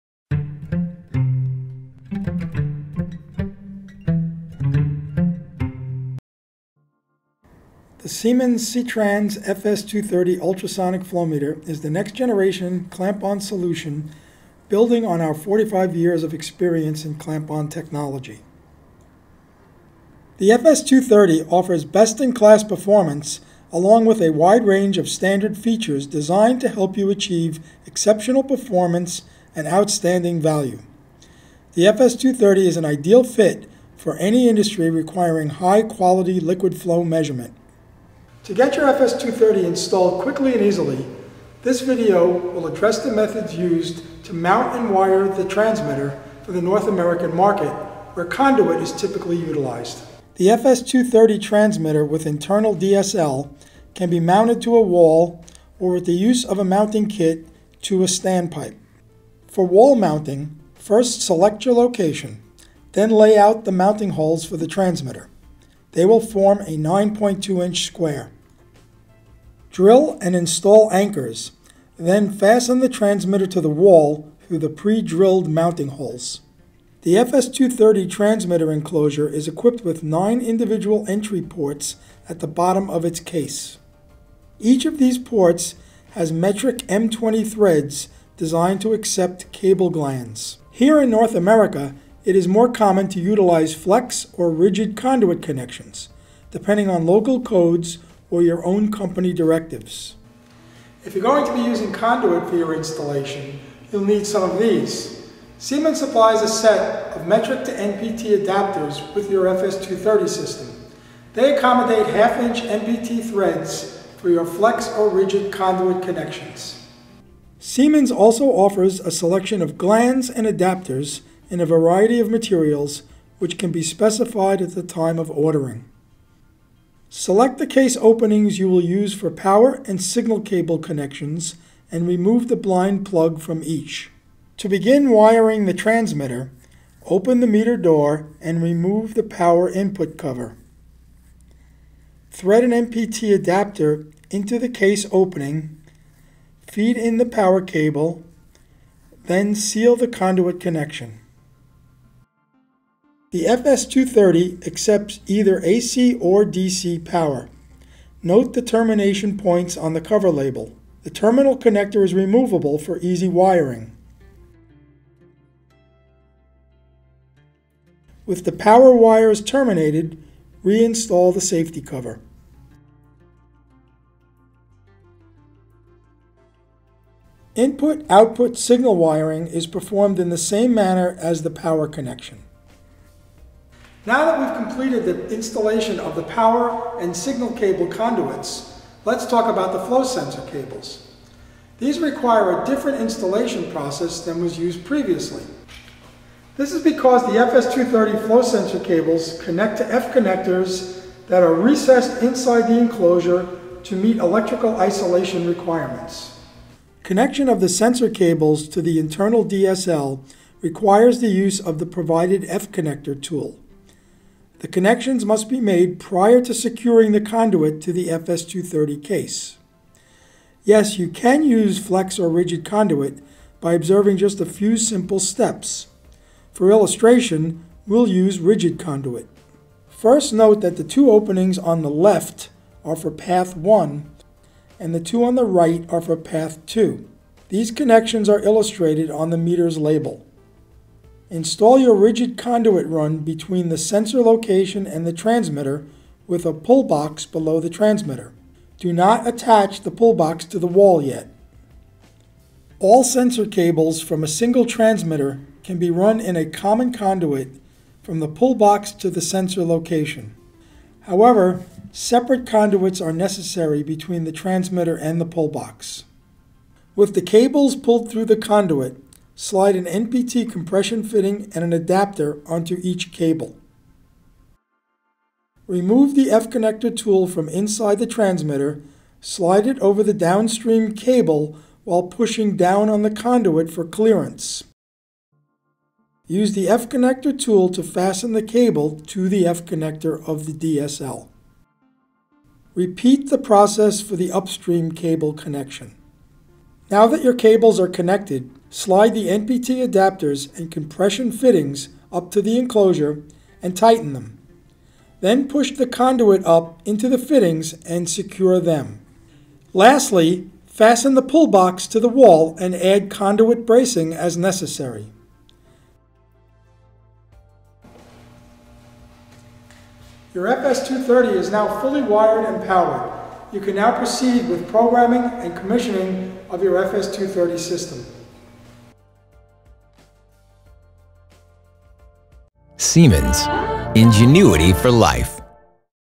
SITRANS FS230 Conduit Installation for the North American Market
Instructions for installing the SITRANS FST030 transmitter (part of the FS230 flow system) where rigid or flex conduit will be utilized for cable installations, as is common in North America.